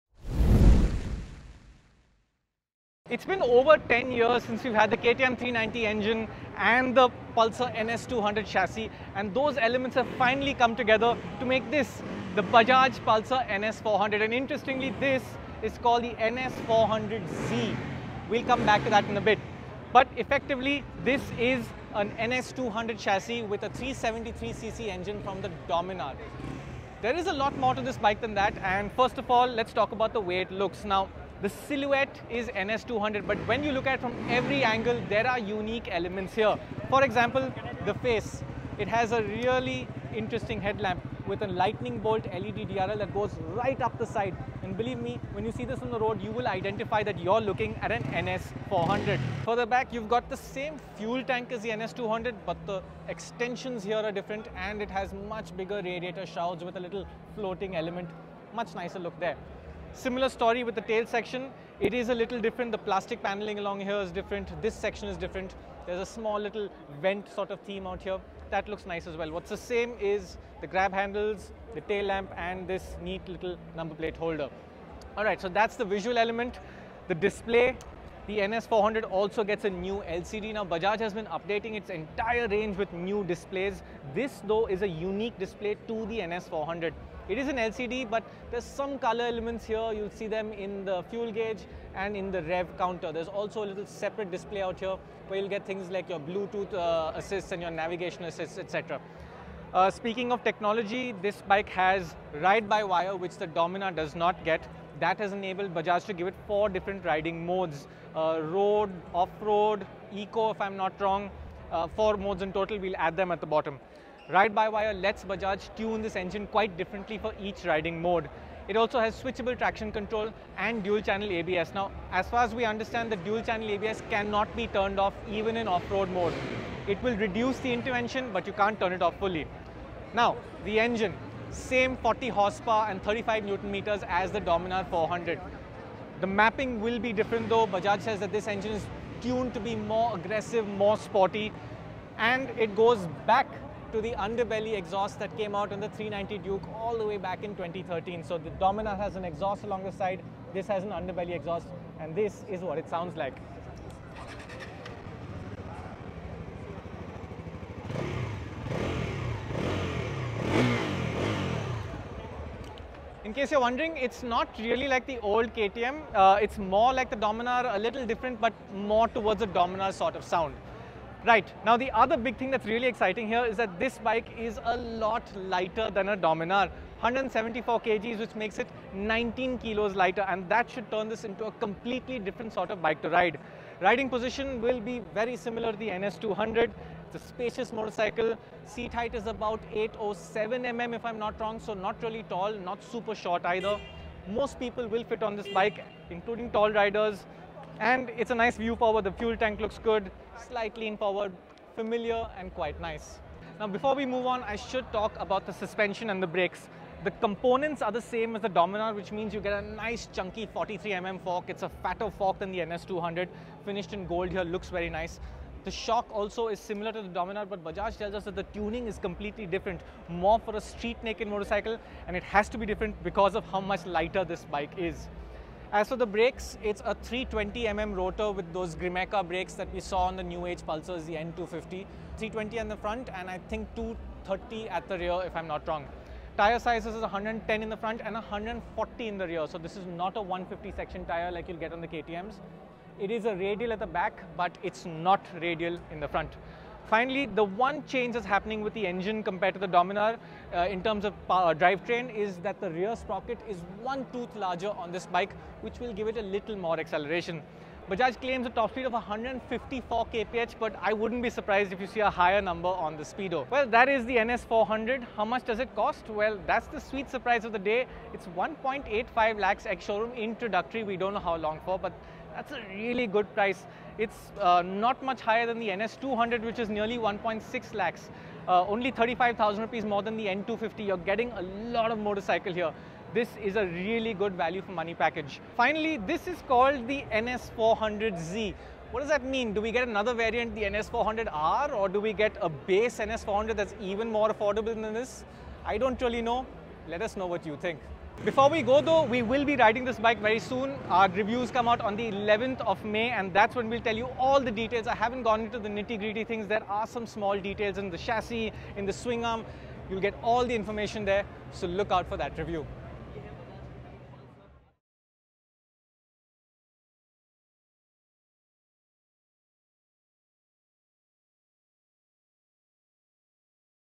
Bajaj Pulsar NS400Z walkaround - Meet the most powerful Pulsar
The biggest Pulsar ever packs strong performance and fantastic value for money. Rishaad Mody tells you more.

Cameraperson - Mrityunjay Chakraborty
Editor - Sanil Sawardekar